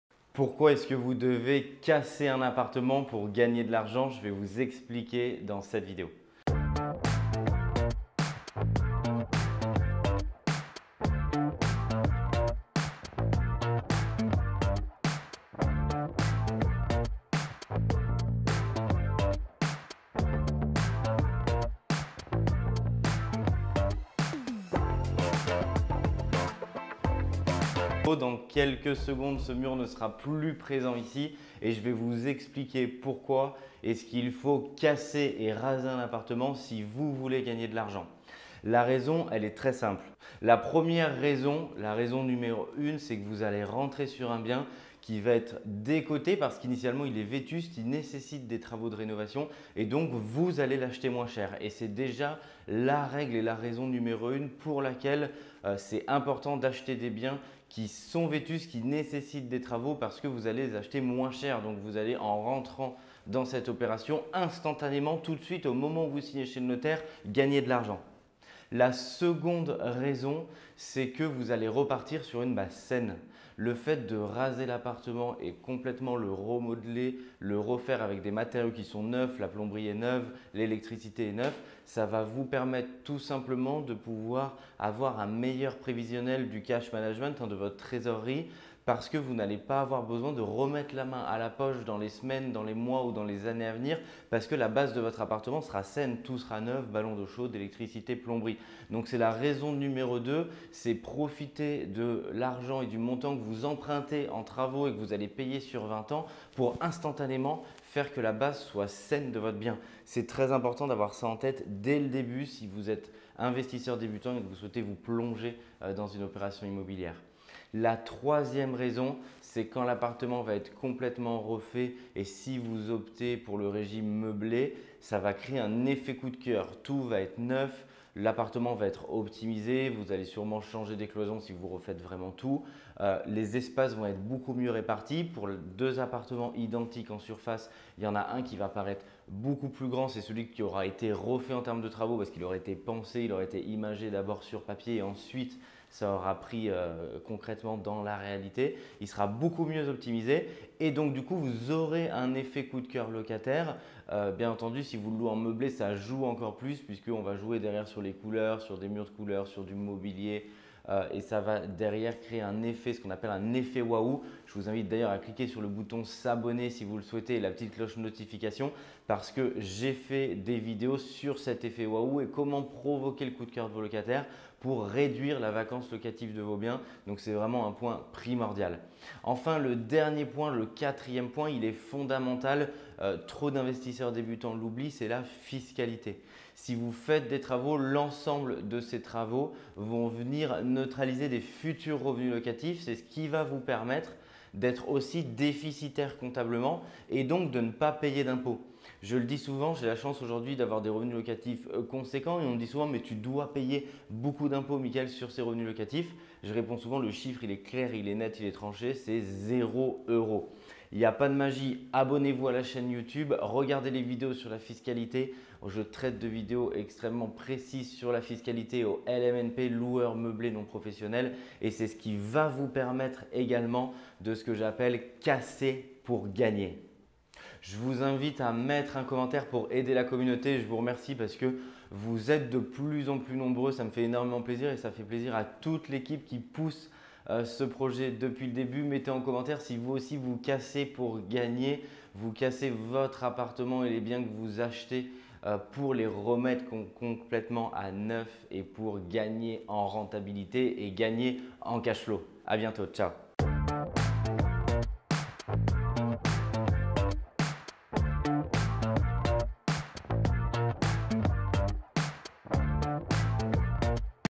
Division Immobilière : Comment Augmenter La Rentabilité De Son Investissement ?
La division immobilière permet d'augmenter la rentabilité de son investissement. Pourquoi est-ce que vous devez casser un appartement pour gagner de l'argent ? Je vais vous l'expliquer dans cette vidéo.

La première raison c'est que vous allez rentrer sur un bien qui va être décoté parce qu’initialement il nécessitera des travaux de rénovations et donc vous allez l'acheter moins cher et c'est la raison pour laquelle c'est important d'acheter des biens qui sont vétustes.

La seconde raison c'est que vous partez sur une base saine. Le fait de raser l'appartement et de le remodeler ça va vous permettre de pouvoir avoir un meilleur prévisionnel du cash management. Donc il faut profiter de l'argent que vous emprunter en travaux pour que la base de votre bien soit saine.

La troisième raison ce que quand l'appartement va être complètement refais, ça va créer un effet coup de cœur. Tout va être neuf, l'appartement va être optimisé. Bien entendu si vous louez en meublé ça joue encore plus.

Bon visionnage.